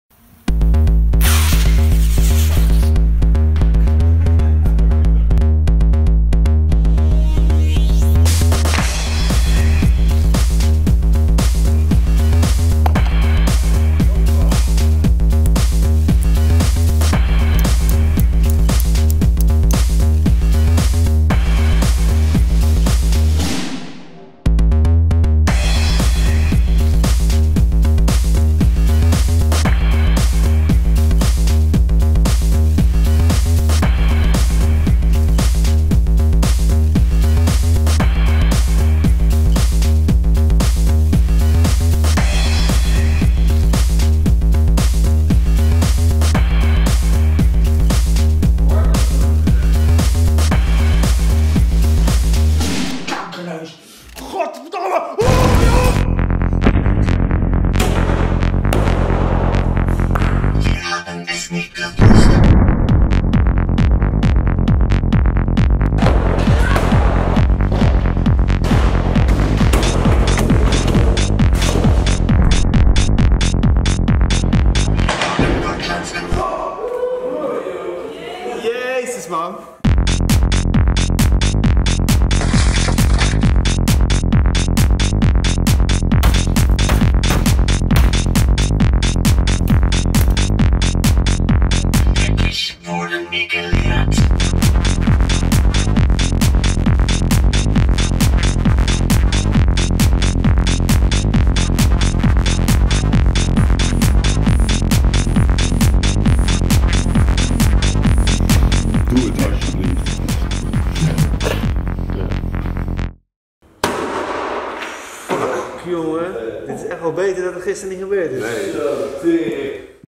What Goes Up - Behind the Scenes (48HFP Rotterdam 2017, OOCHOO)
Behind the scenes of What Goes Up. Made for the 48 Hour Film Project Rotterdam 2017 by OOCHOO.